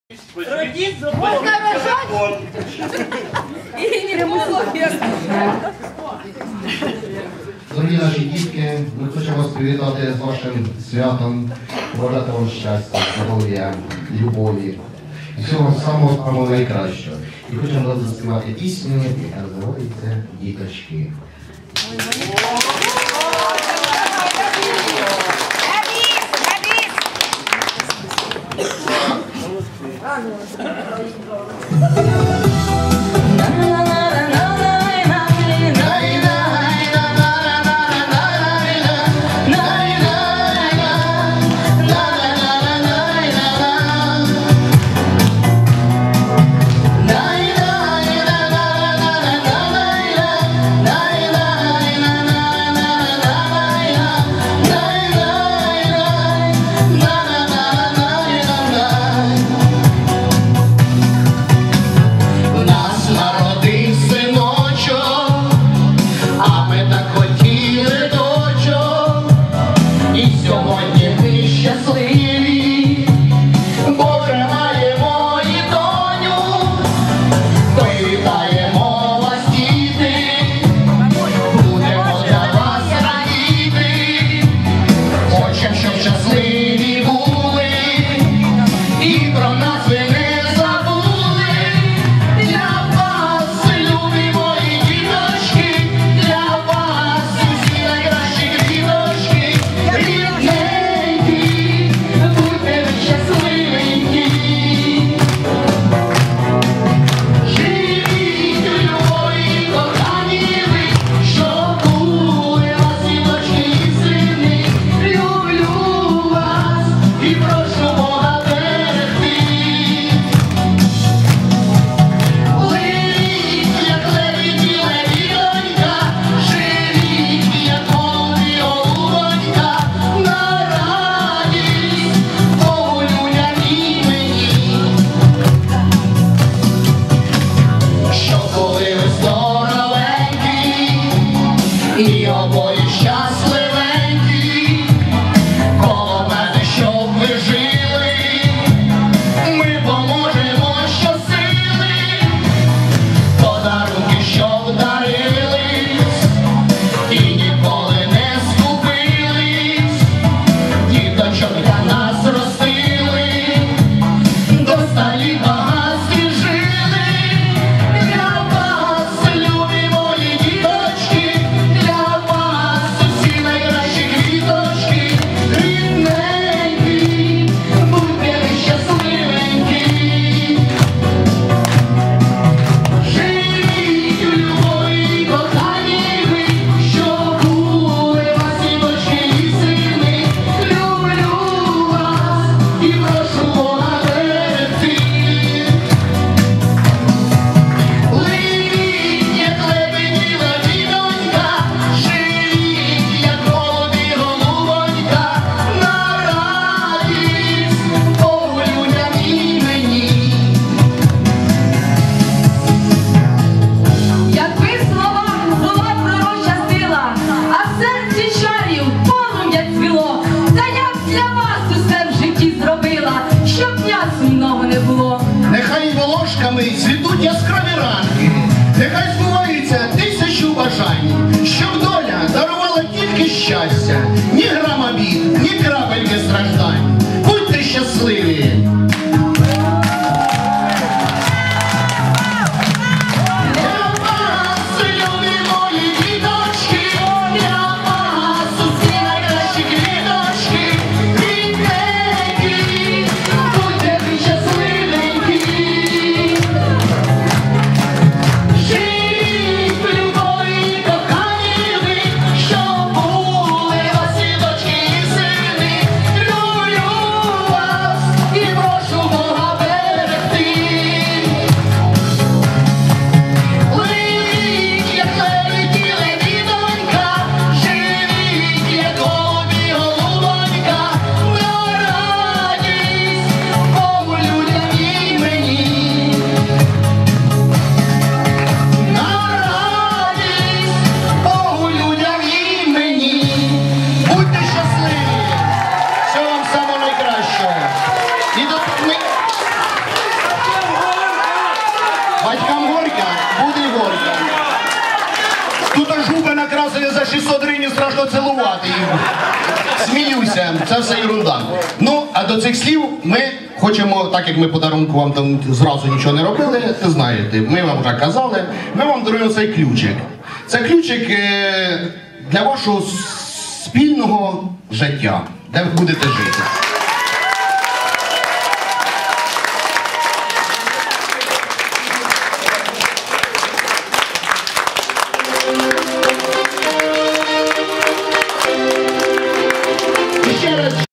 Свадьба
Песня для молодых ,поют родители жениха .
Пісня для молодих, співають батьки нареченого.
A song for the young, the groom's parents sing.